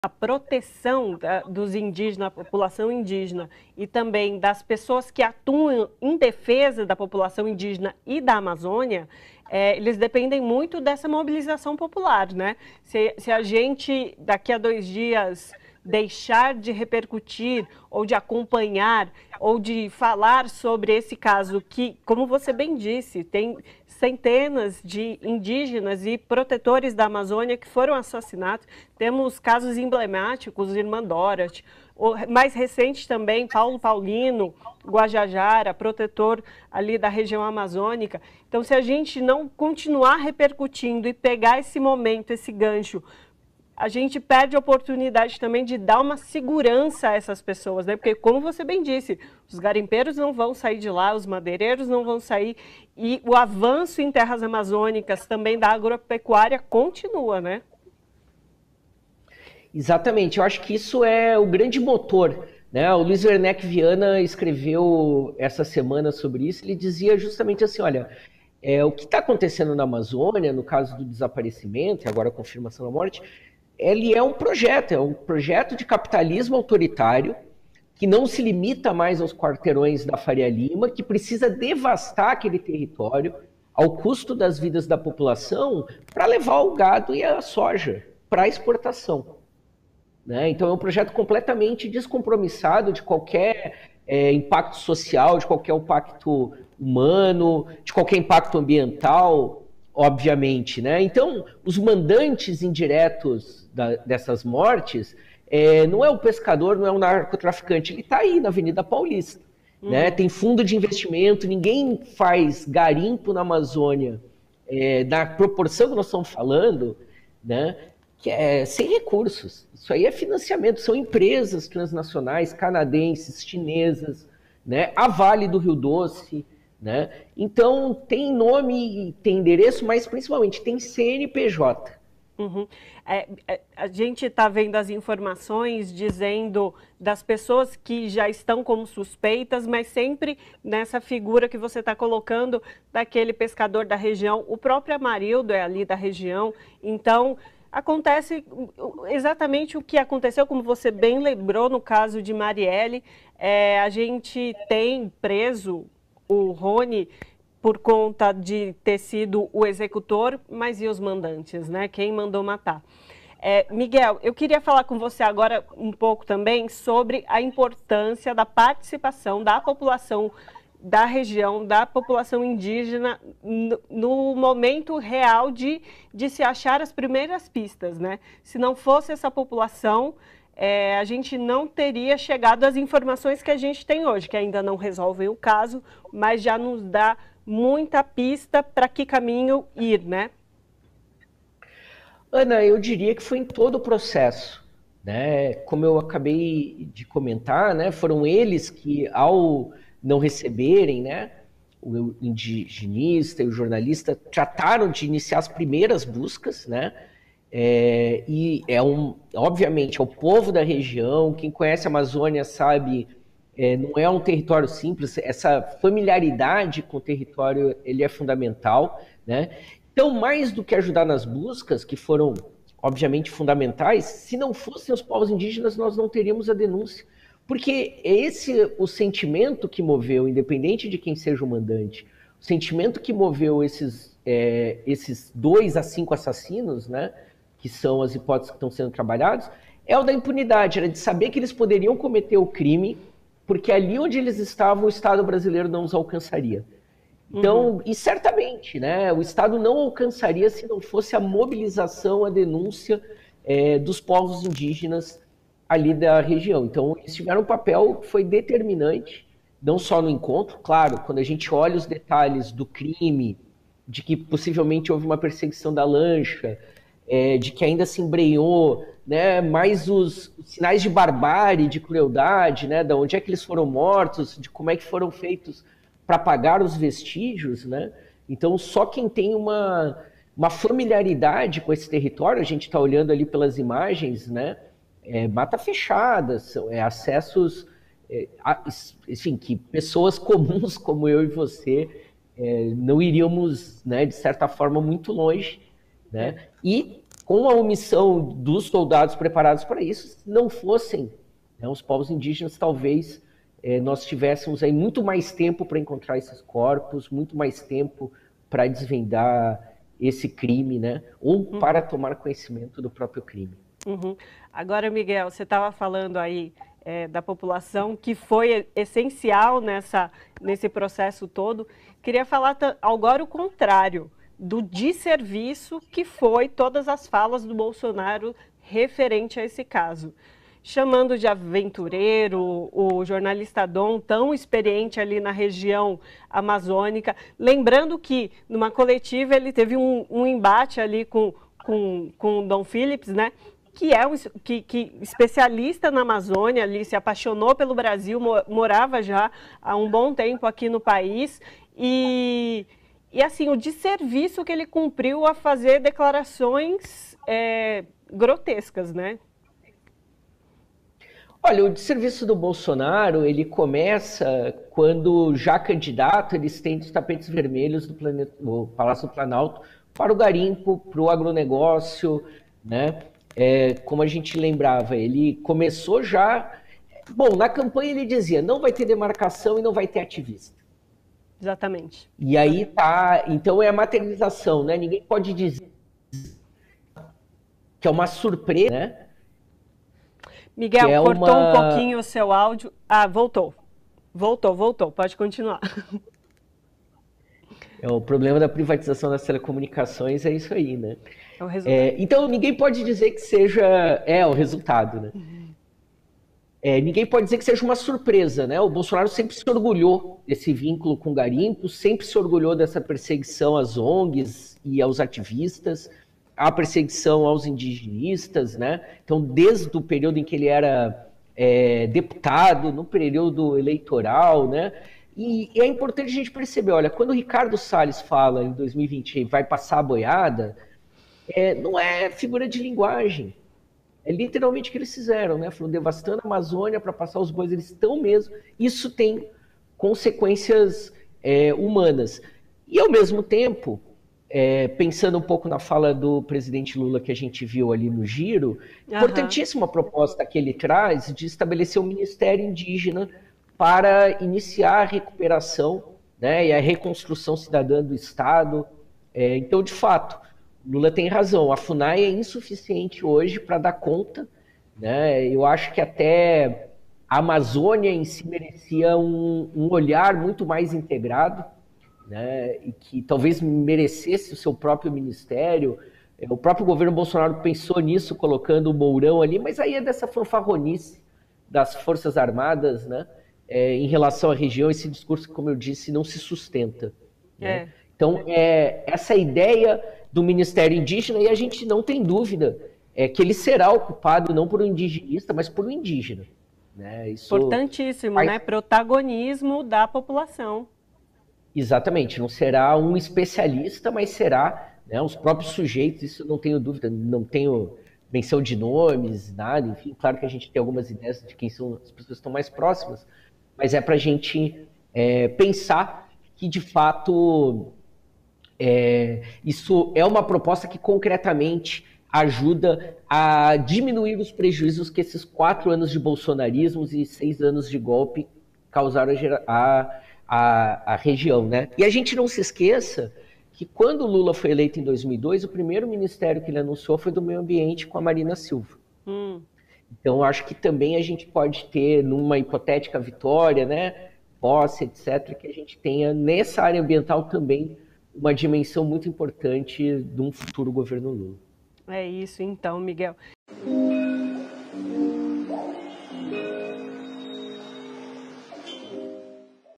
O que está acontecendo na Amazônia é um projeto de capitalismo autoritário
Miguel Stedile,, Editor do Ponto Newsletter do Brasil de Fato, fala sobre a escala de crimes contra os povos indígenas e defensores de direitos humanos na Amazônia.